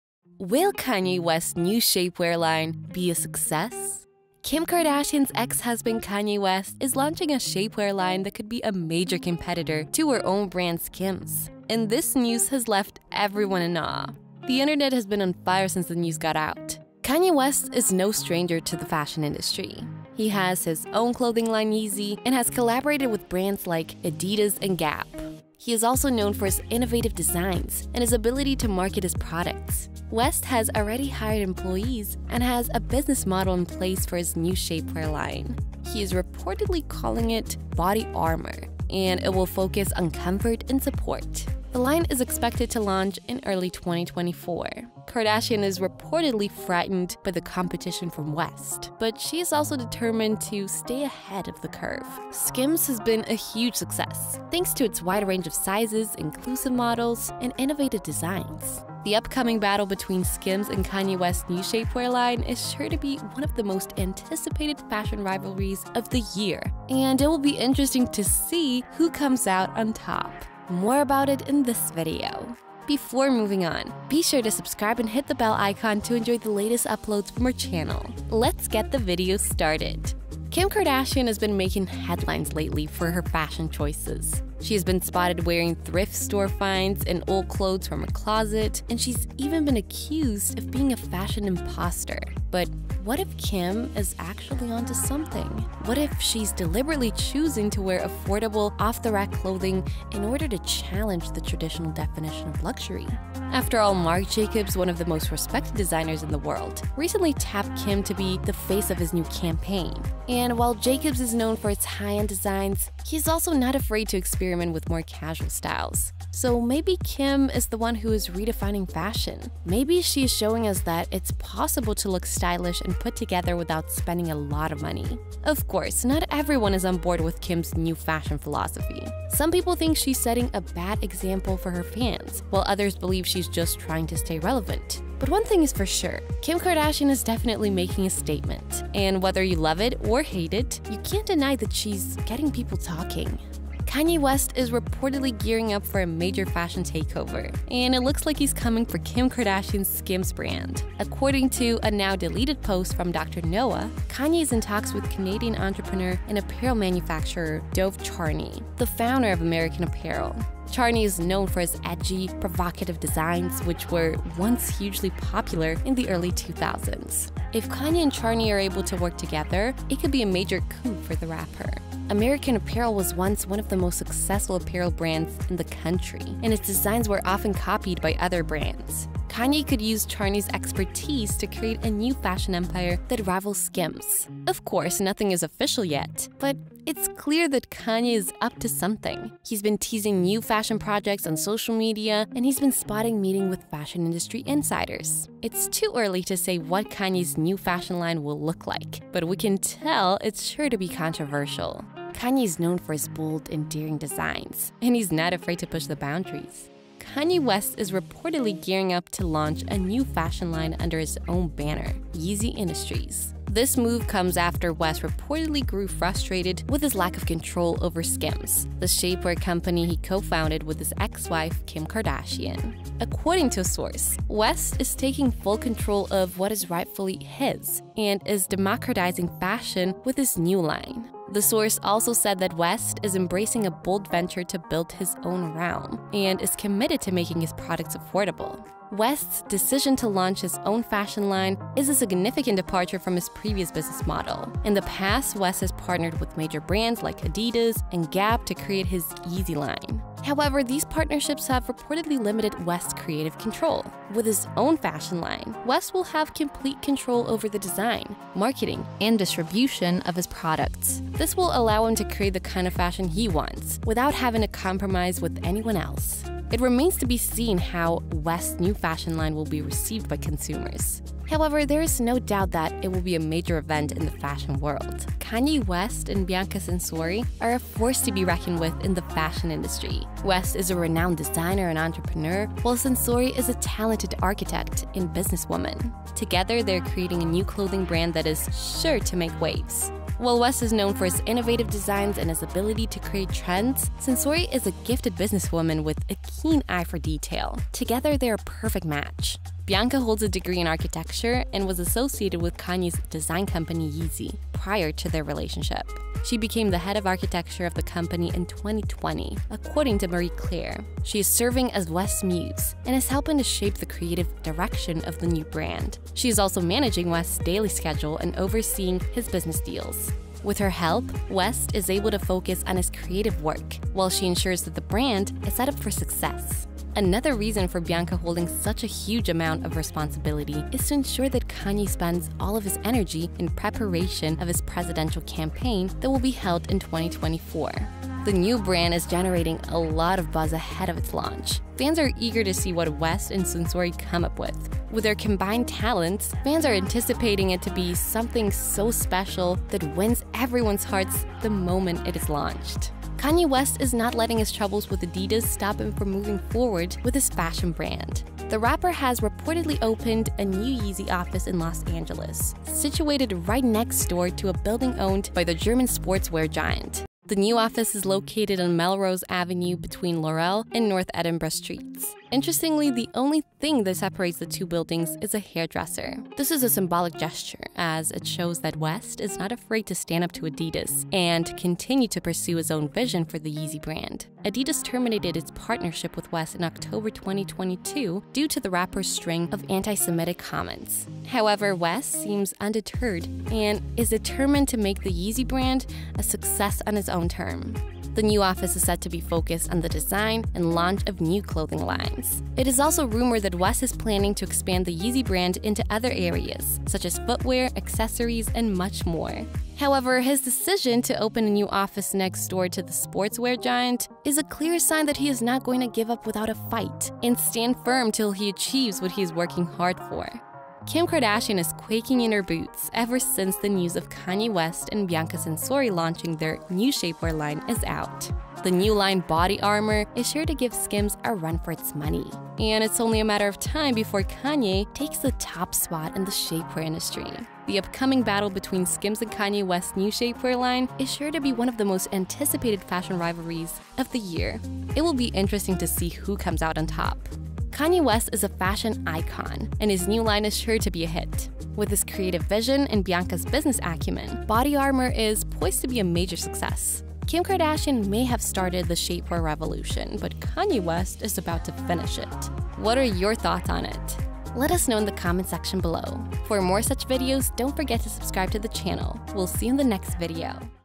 KIM KARDASHIAN PANICS AS KANYE AND BIANCA LAUNCH SKIMS RIVAL BRAND
Will Kanye West's new shapewear line be a success?

Kim Kardashian's ex-husband, Kanye West, is launching a shapewear line that could be a major competitor to her own brand, Skims. And this news has left everyone in awe! The internet has been on fire since the news is out.

More about it in this video. Let's get the video started!